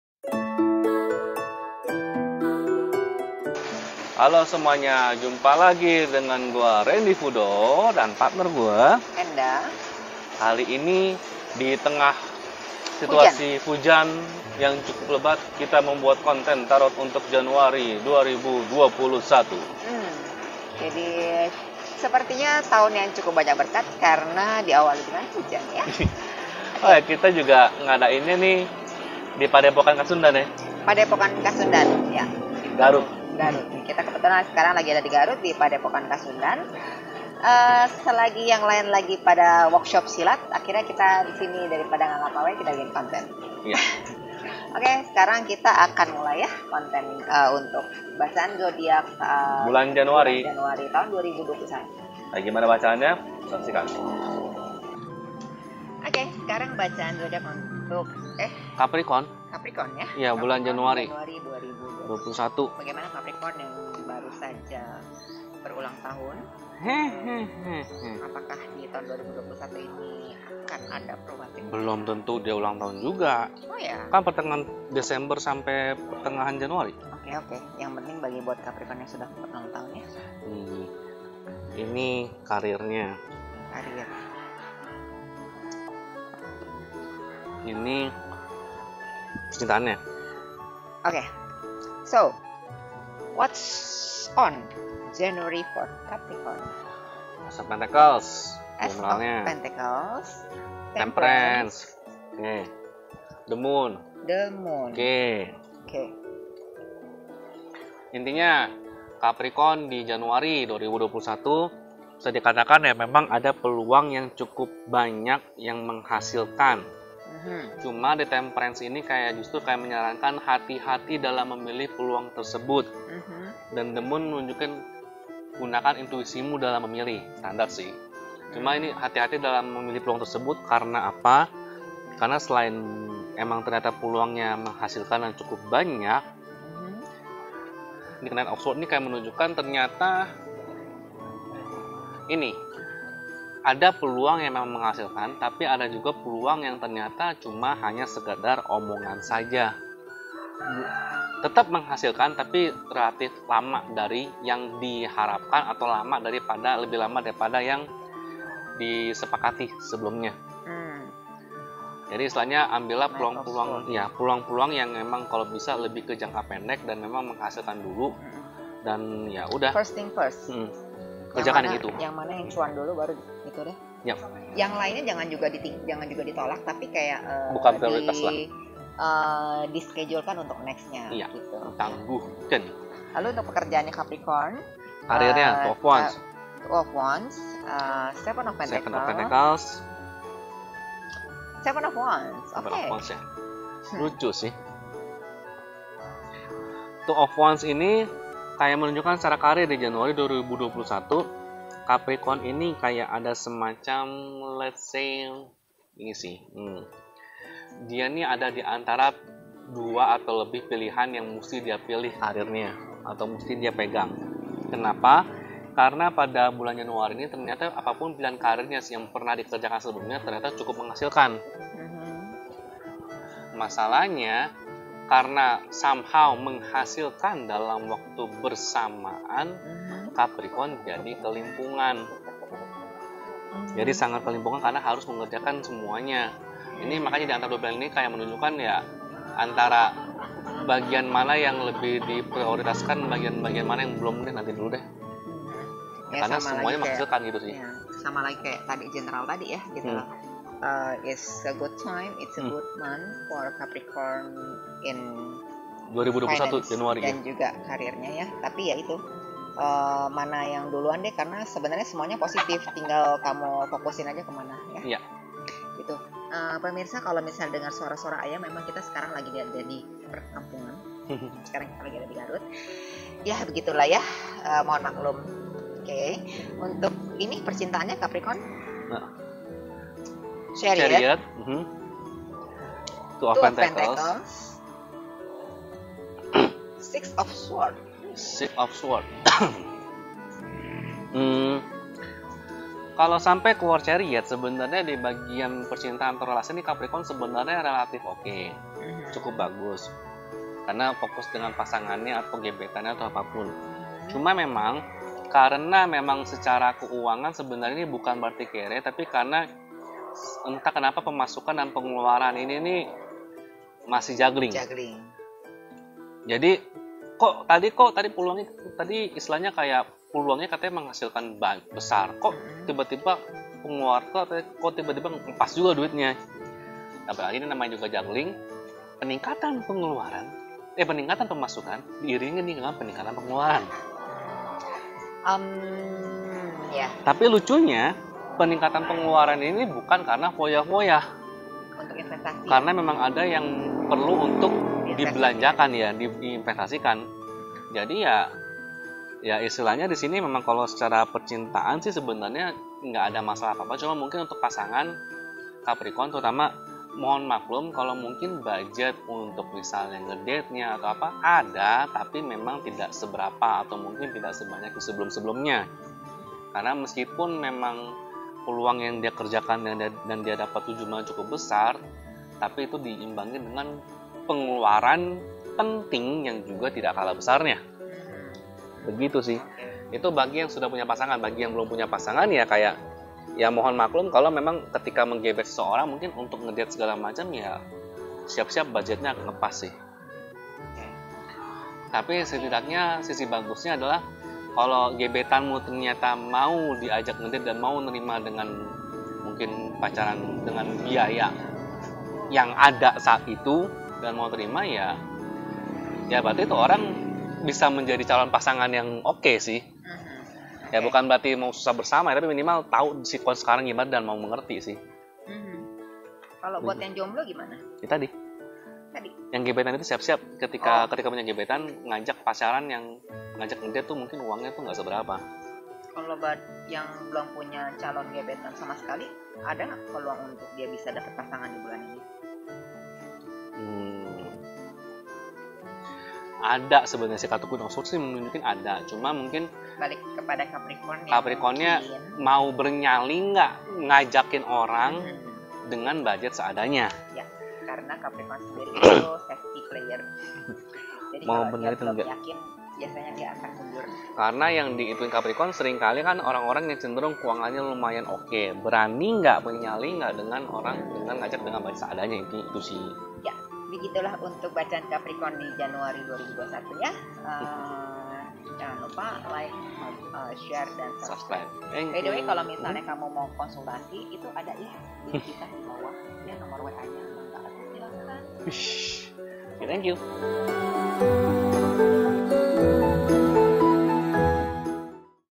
Ramalan Karir dan Cinta Zodiak Capricorn Januari 2021 : Ada Pengeluaran Uang Untuk Investasi
Berikut ramalan zodiak untuk bulan Januari 2021. Semoga resonet dan bermanfaat yah :-)

Untuk syarat dan ketentuan dalam konsultasi tarot, bisa WhatsApp di

@rendyfudoh (general)
@RFTarot (untuk khusus tarot)